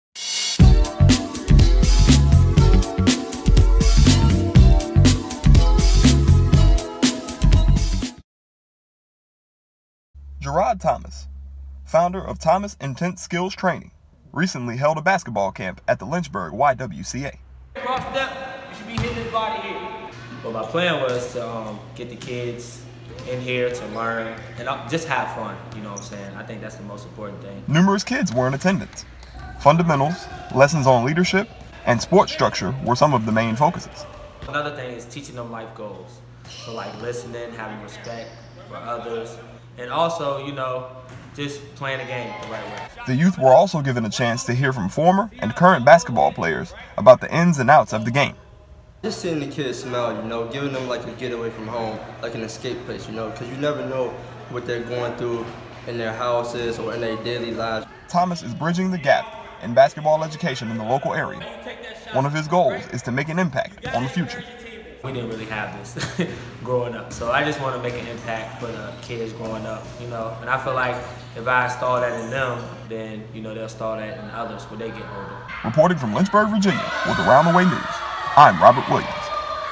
Around The Way News: Episode 10: Thomas Intense Skills Training Basketball Camp
ATWN's own Robert Williams got an inside look into basketball trainer Jarrod Thomas' basketball camp at the Lynchburg YWCA.